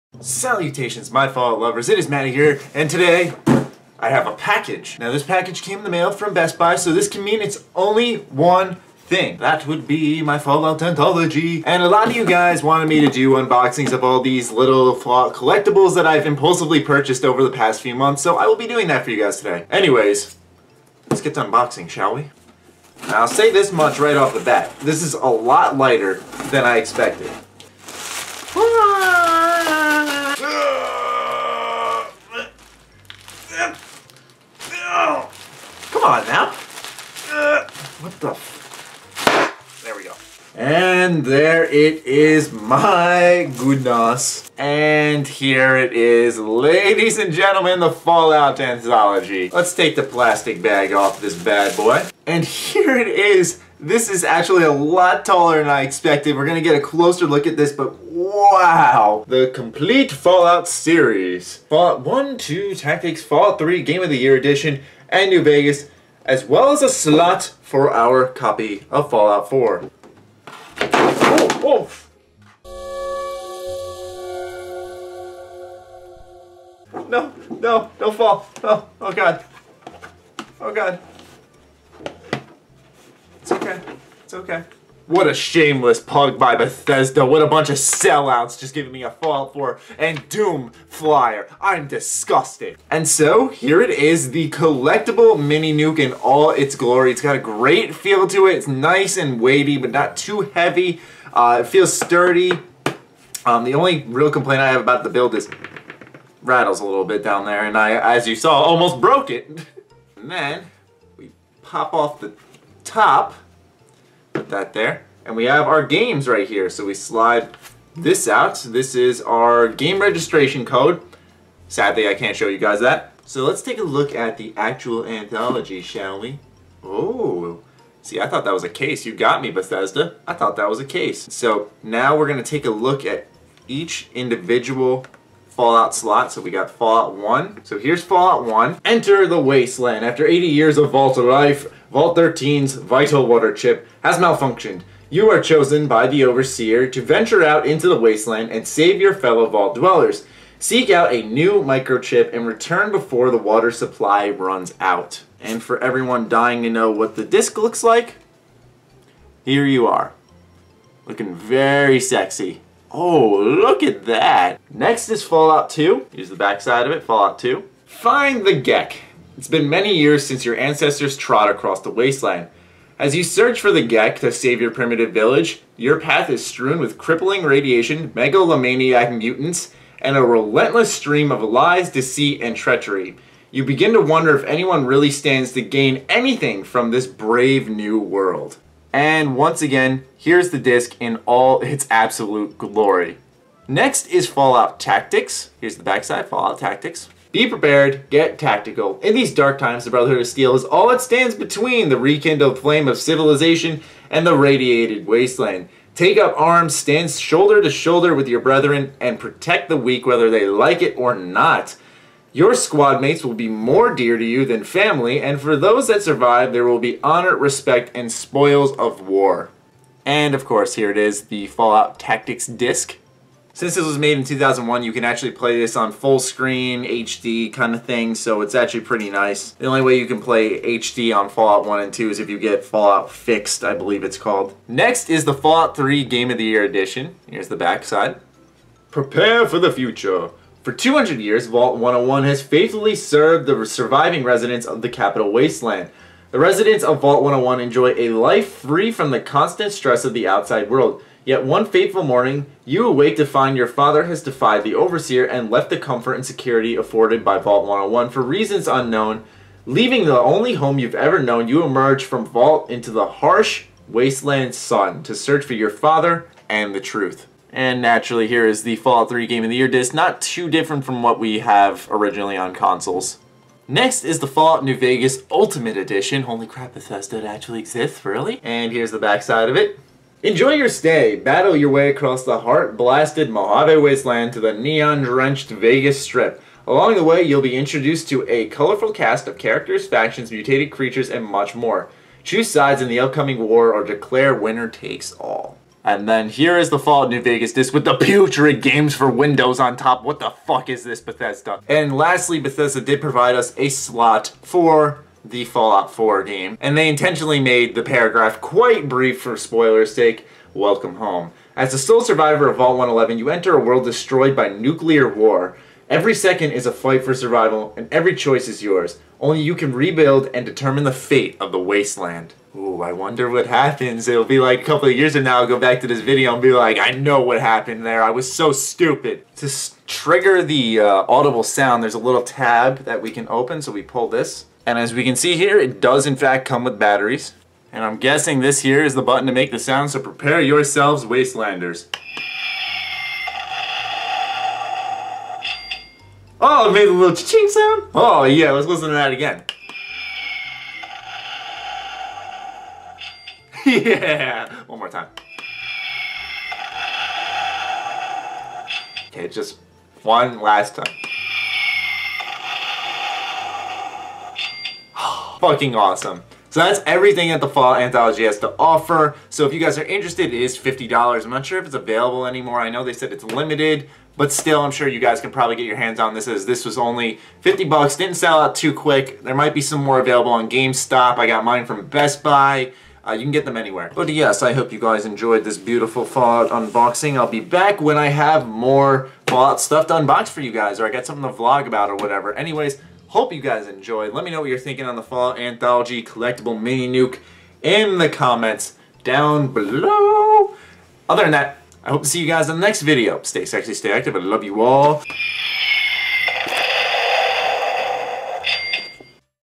FALLOUT ANTHOLOGY UNBOXING!
IT'S UNBOXING SEASON! Can't wait to unbox the rest of the impulsive Fallout purchases for y'all.